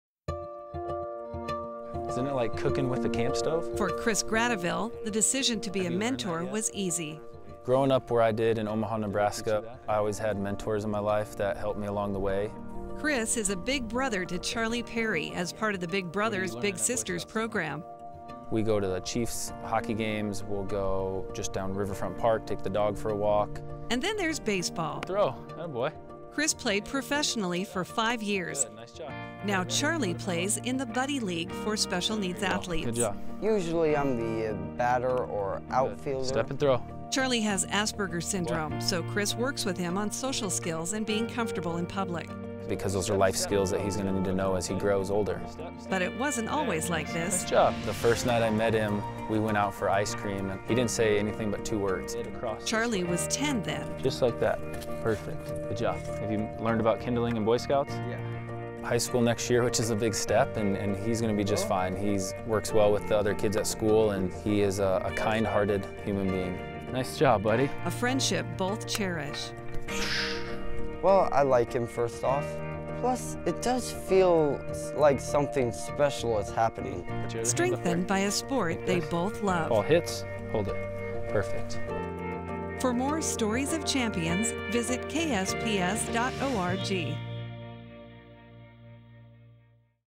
Chris Gradoville, Big Brother and American Graduate Champion
Chris Gradoville is a former Spokane Indian player, and a big brother to Charlie Perrey, as part of the Big Brothers and Big Sisters program.This story will be shared with the nation on PBS during American Graduate Day special, October 14, 11-3 on KSPS-TV in Spokane.